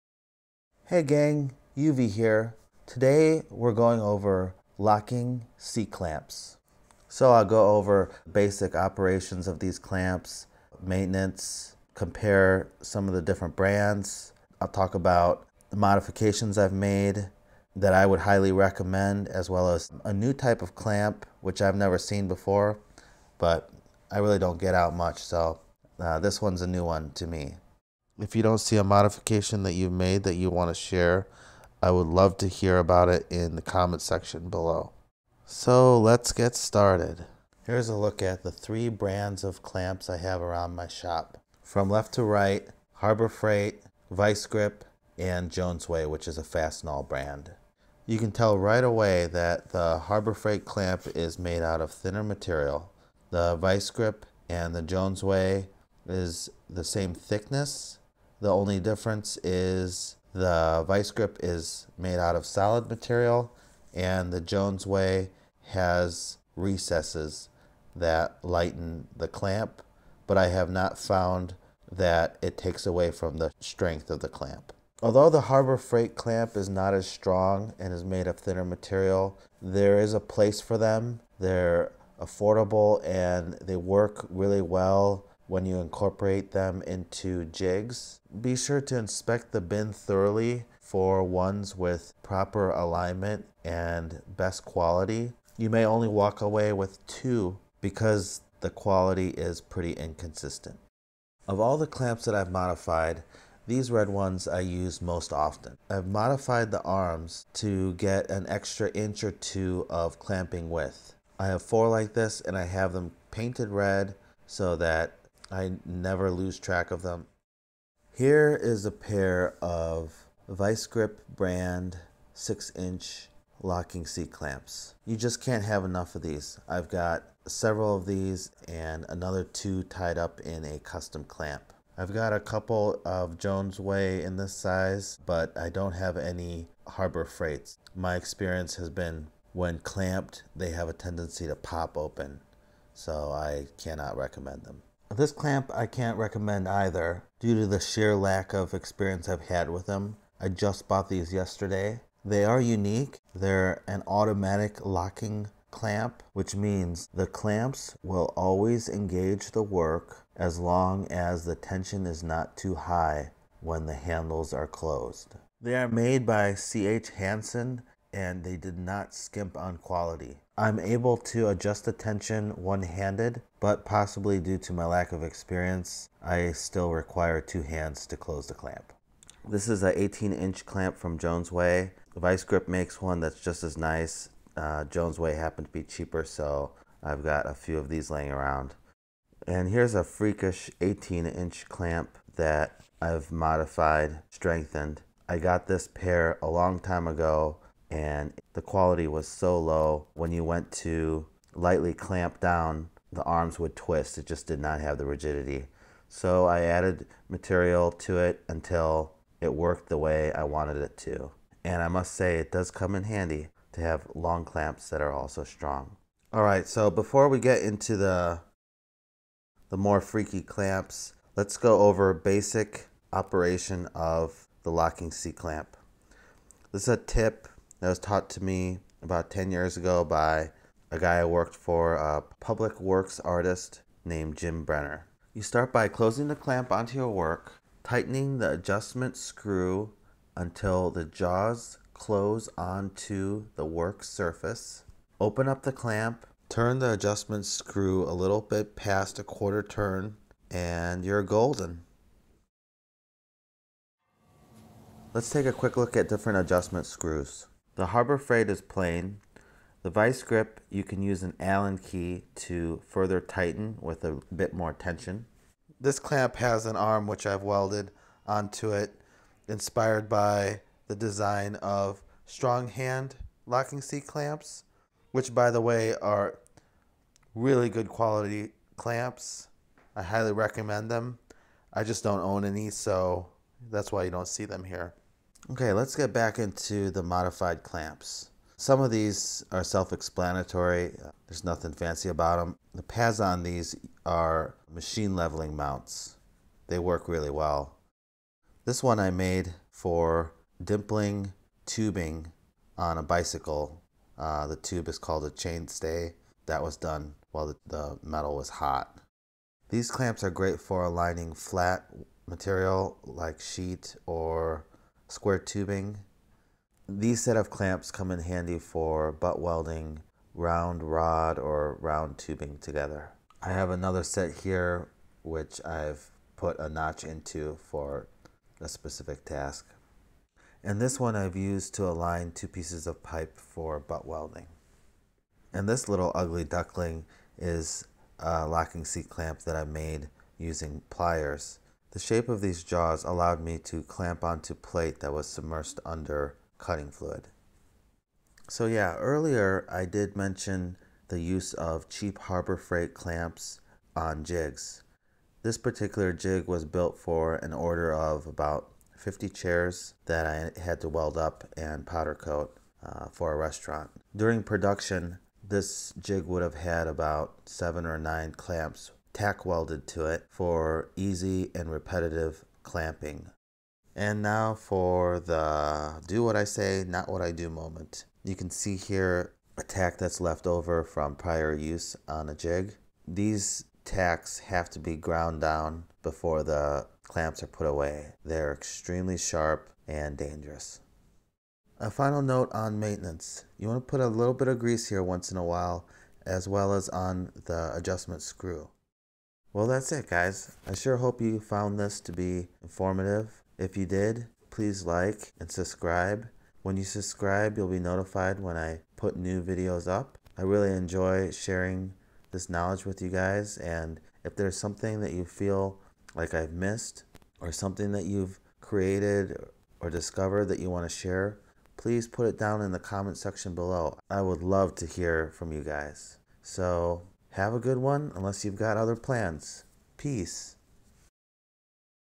Locking c-clamps
We go on a locking c-clamp adventure, where I cover modification, maintenance, and operation.  Also, a look at different styles and compare clamps I own by manufacturer.